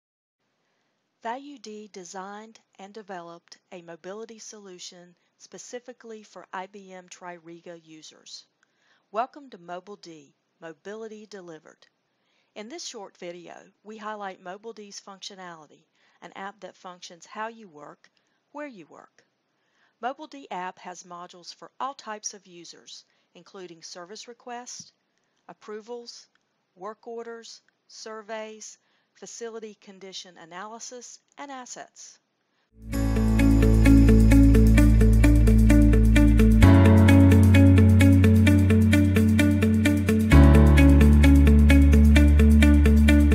MobilD Demo – An IBM TRIRIGA Mobile App - Series Part 1
ValuD continues to demonstrate its expertise in IBM TRIRIGA with the successful launch of its TRIRIGA driven mobility solution "MobilD".

MobilD – An IBM TRIRIGA Mobile Application - Evolving Beyond Mobile Browsers.